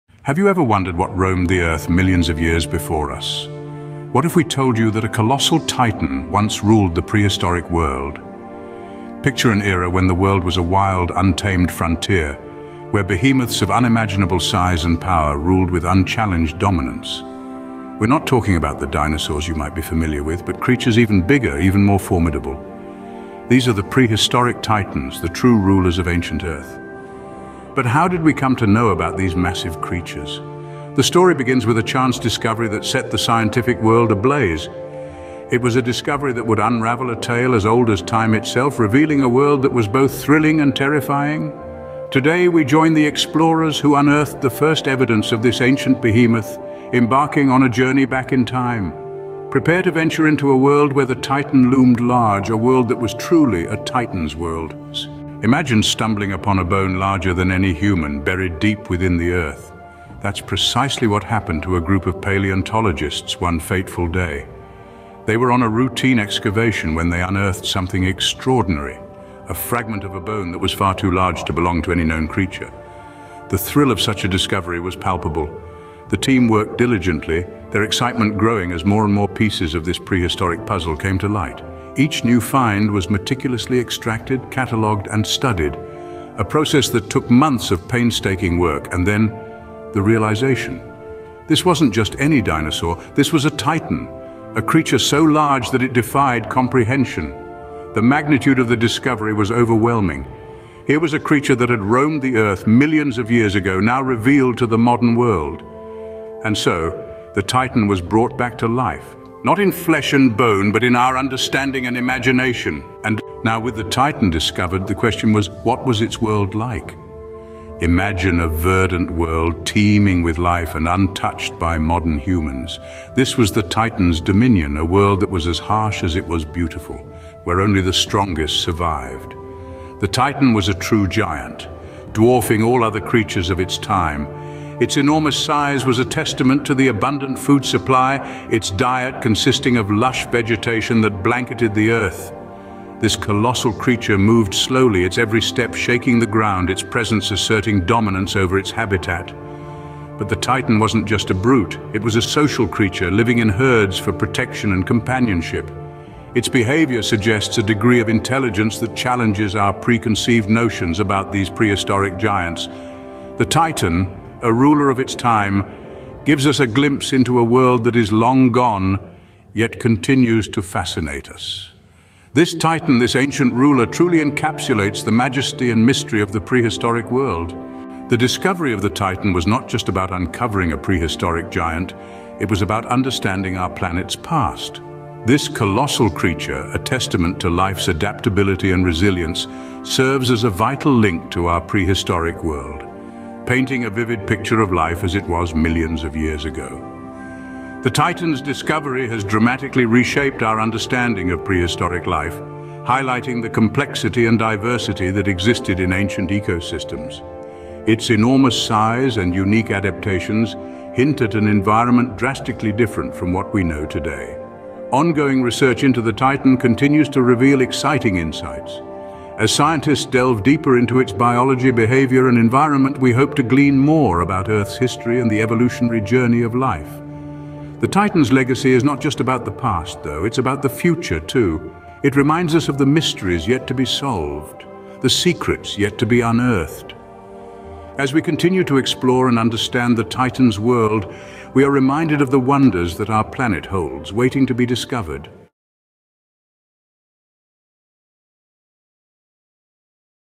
Unveiling the Titan: A Prehistoric Giant
Embark on an awe-inspiring odyssey as we unveil the unearthing of a colossal prehistoric behemoth. Our expedition delves into the sheer magnitude, habitat, and profound significance of this ancient titan in the annals of history. Towering over its epoch, this majestic creature's size alone is a testament to a bygone era's grandeur, reshaping our understanding of the Earth's primordial past. Journey alongside us as we piece together its habitat, envisioning a world where such colossal beings roamed untamed landscapes. This discovery serves as a window into the enigmatic world that predates our own, shedding light on the interconnected web of life that thrived in a time long forgotten. Join this extraordinary endeavor as we unravel the mysteries and importance of this prehistoric giant, bridging the gap between the ancient and modern realms of scientific discovery.